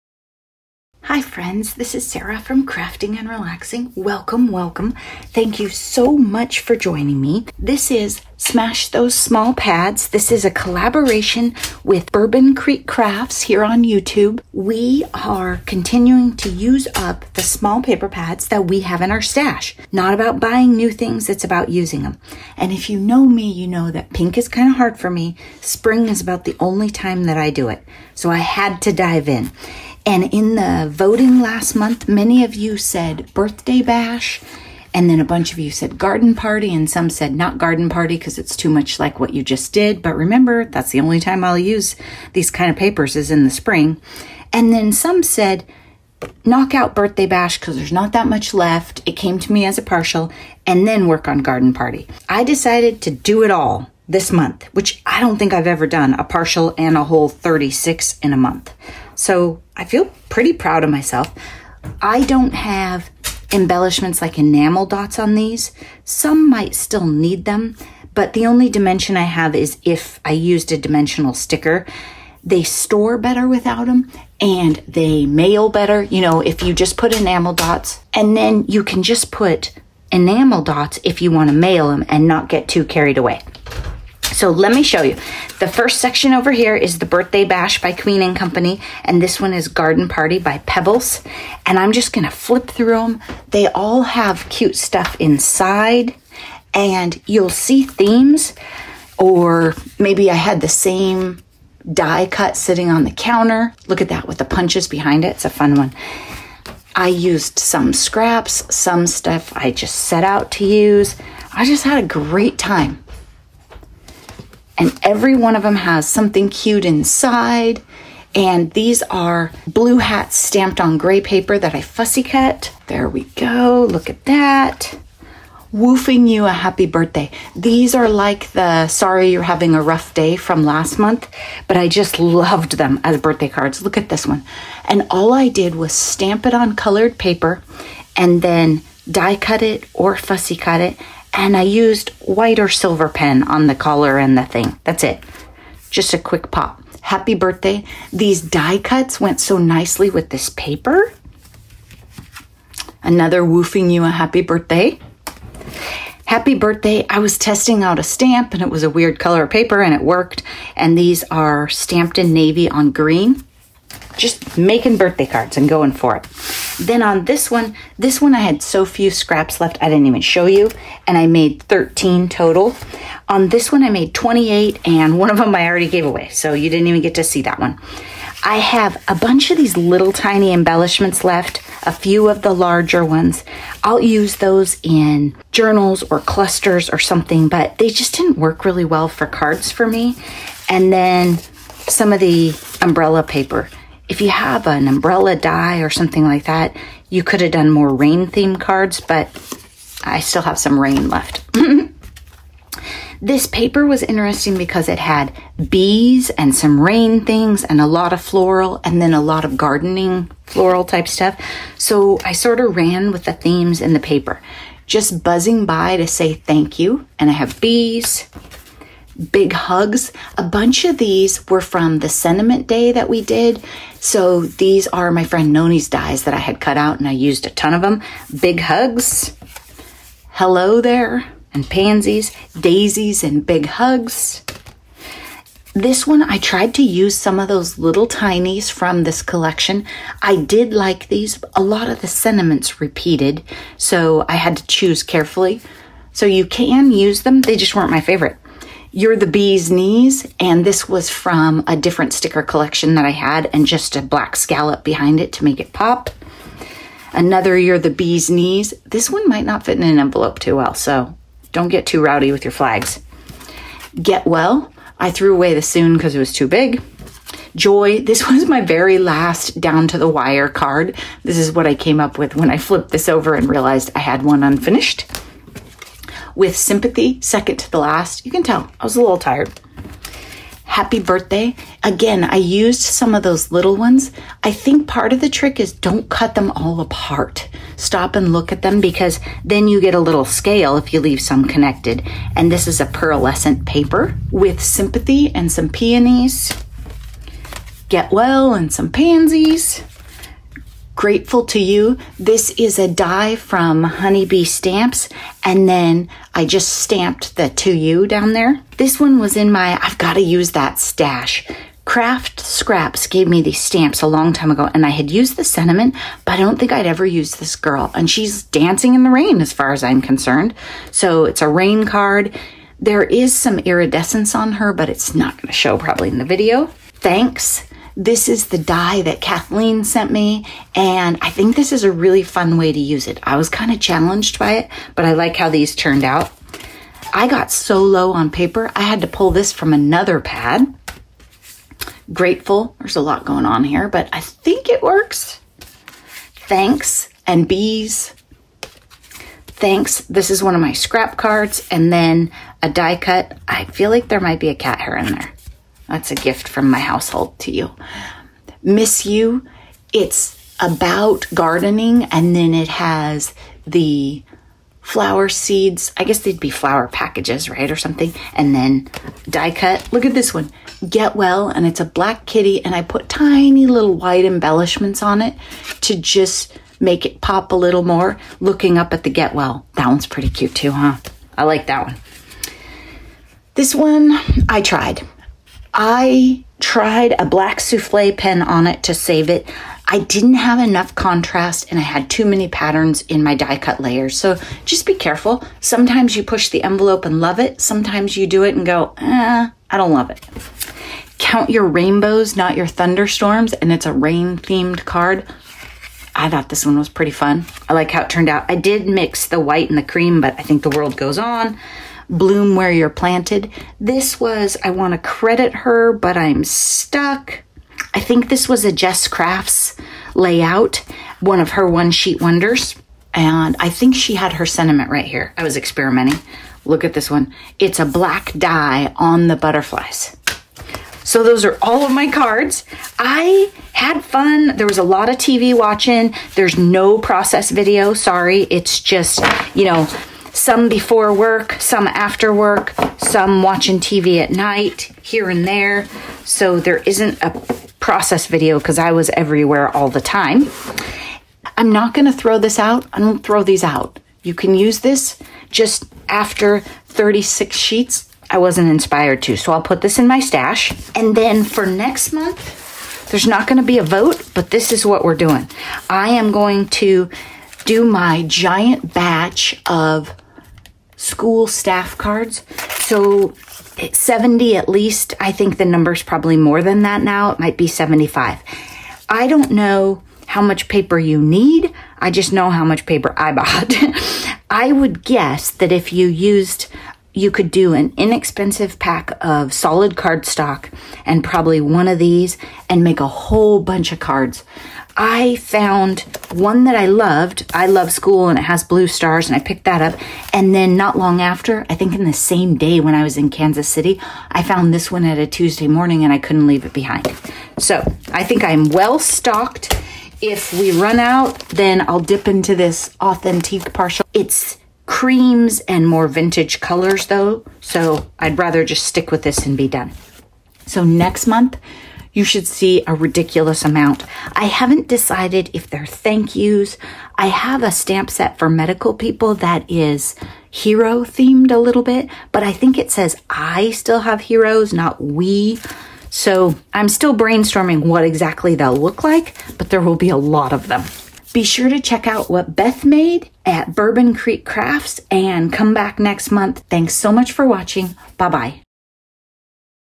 Smash Those Small Pads - March - cards from 2 6x6 Pads!
Smash Those Small Pads is a collaboration I'm doing with @BourbonCreekCrafts to use up our small paper pads! Join us at the end of each month to see what paper pad we used and what we created with it!!

This month I used up TWO 6x6 paper pads!!

I appreciate every comment and thumbs up! Is there someone you could share my video with? That would be fabulous!

Love my projects, but don't want to make your own?